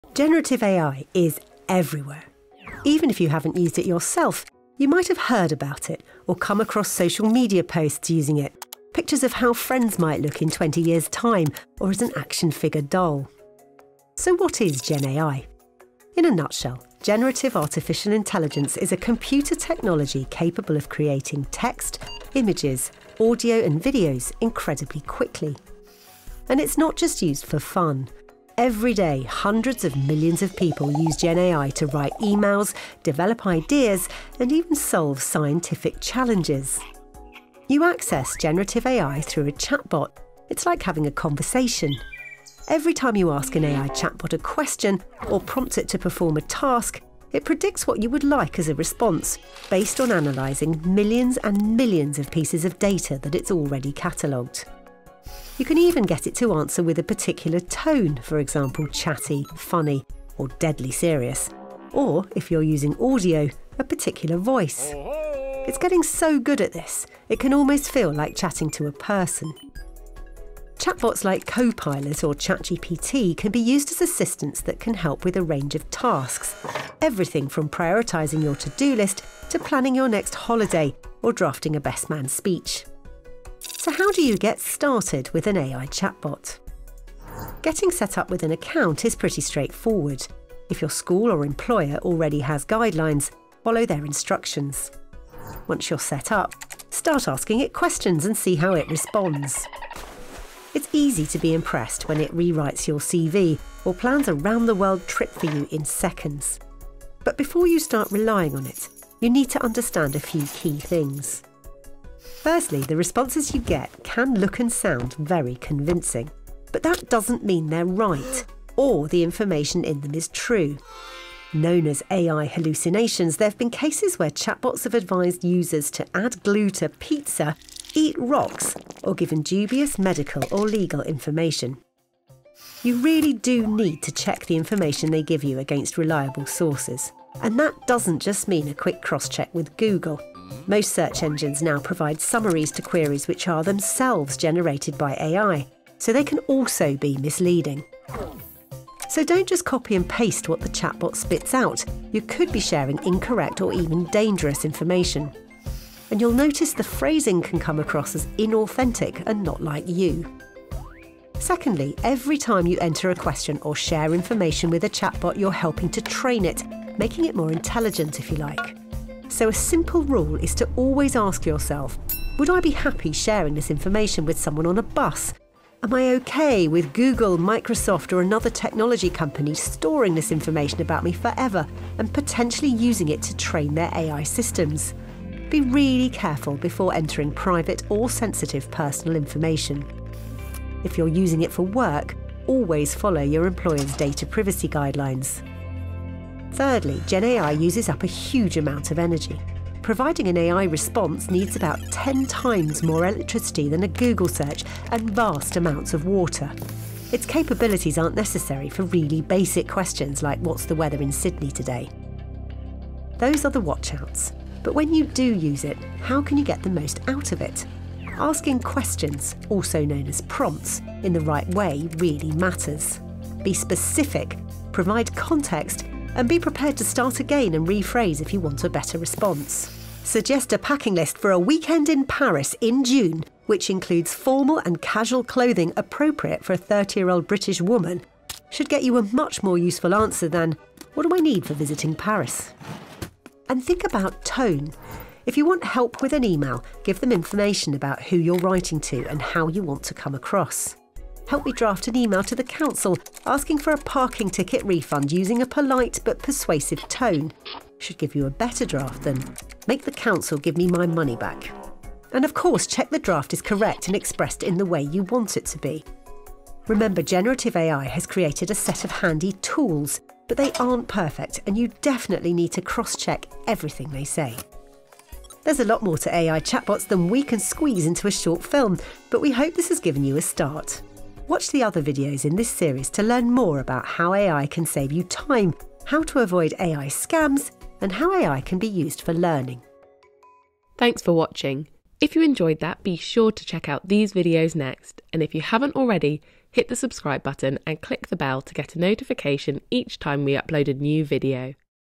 What are AI chatbots? | BBC Ideas
Curious about AI but not sure where to start? Here's a handy guide from the BBC's tech editor Zoe Kleinman.

Watch the other videos in our series on AI

Animated by OOF Animation

00:00 Introduction to generative AI
00:37 Gen AI and chatbots
01:23 How to get started with an AI chatbot
02:00 AI hallucinations and double checking chatbot answers
02:41 Being careful about what you share with a chatbot
03:16 AI and energy use
03:33 How to get the most out of chatbots
04:26 Conclusions